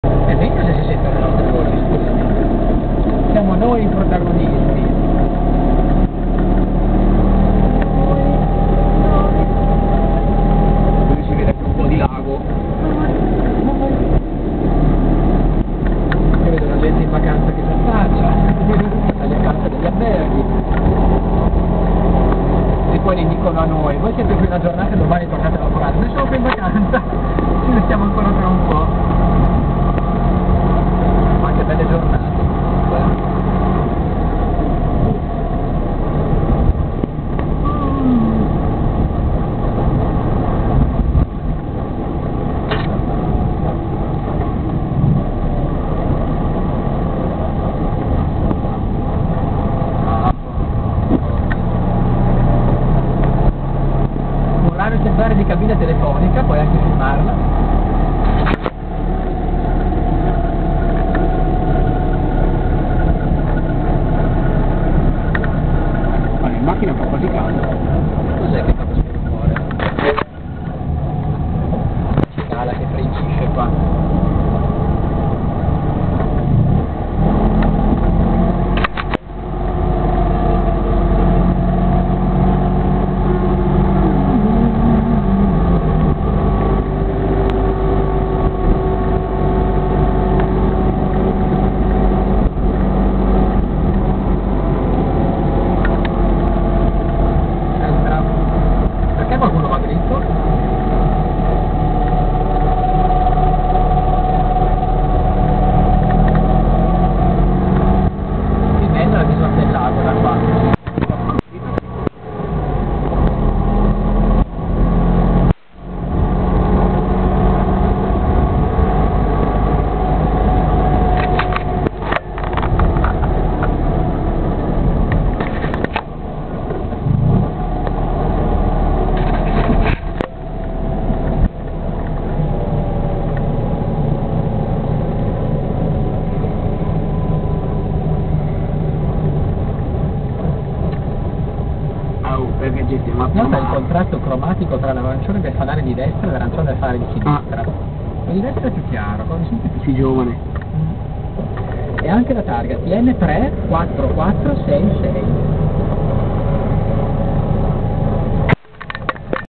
Fiat 500 a Sirmione 2° parte.
IT: Raduno a Sirmione del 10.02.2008, seconda parte.
EN: Old car meeting in Sirmione, Italy.